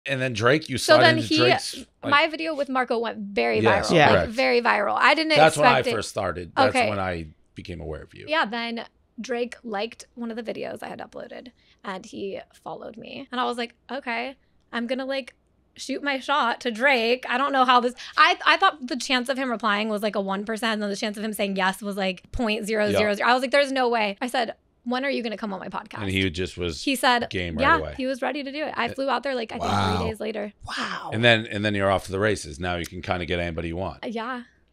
How Did Bobbi Althoff Get Drake To Come On Her Podcast?
Crazy story to get Drake on your 4th ever episode